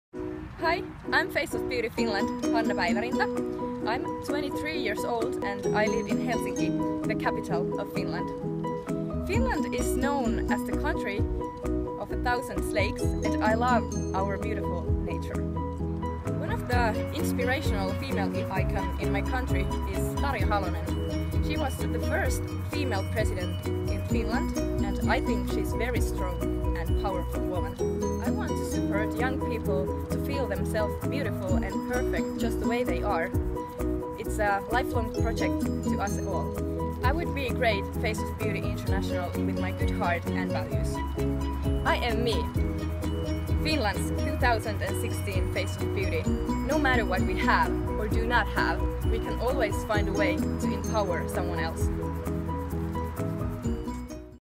Face of Beauty Finland 2016 - I AM ME Video
I AM ME, Finland's 2016 Face of Beauty, no matter what we have or do not have, we can always find a way to empower someone else.

"I would love to win the title of Face of Beauty International, compete in show jumping, and create big charity projects to help people" - Maria Paivarinta